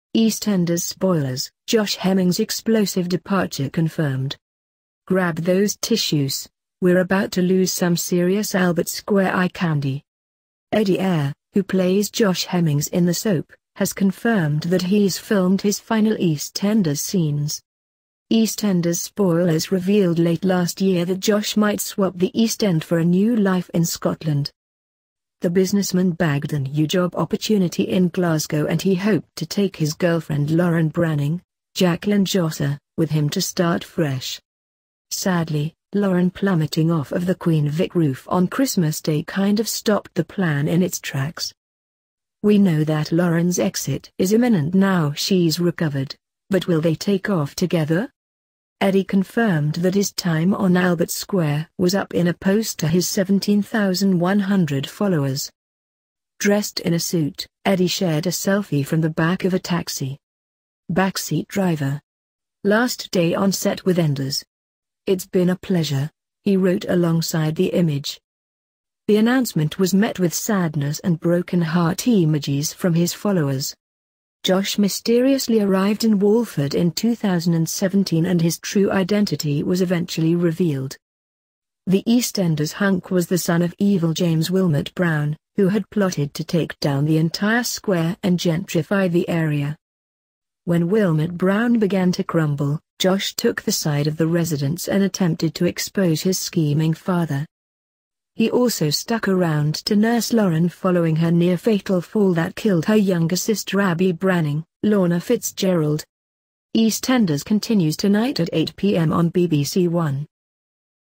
EastEnders spoilers: Josh Hemmings' explosive departure confirmed
GRAB those tissues, we're about to lose some serious Albert Square eye candy.

Eddie Eyre, who plays Josh Hemmings in the soap, has confirmed that he's filmed his final EastEnders scenes.

EastEnders spoilers revealed late last year that Josh might swap the East End for a new life in Scotland.

The businessman bagged a new job opportunity in Glasgow and he hoped to take his girlfriend Lauren Branning (Jacqueline Jossa) with him to start fresh.

Sadly, Lauren plummeting off of the Queen Vic roof on Christmas Day kind of stopped the plan in its tracks.

We know that Lauren's exit is imminent now she's recovered, but will they take off together?

Eddie confirmed that his time on Albert Square was up in a post to his 17,100 followers.

Dressed in a suit, Eddie shared a selfie from the back of a taxi.

"Backseat driver! Last day on set with Enders. It's been a pleasure," he wrote alongside the image.

The announcement was met with sadness and broken heart emojis from his followers.

Josh mysteriously arrived in Walford in 2017 and his true identity was eventually revealed.

The EastEnders hunk was the son of evil James Willmott-Brown, who had plotted to take down the entire Square and gentrify the area.

When Willmott-Brown began to crumble, Josh took the side of the residents and attempted to expose his scheming father.

He also stuck around to nurse Lauren following her near-fatal fall that killed her younger sister Abi Branning (Lorna Fitzgerald).

EastEnders continues tonight at 8pm on BBC1.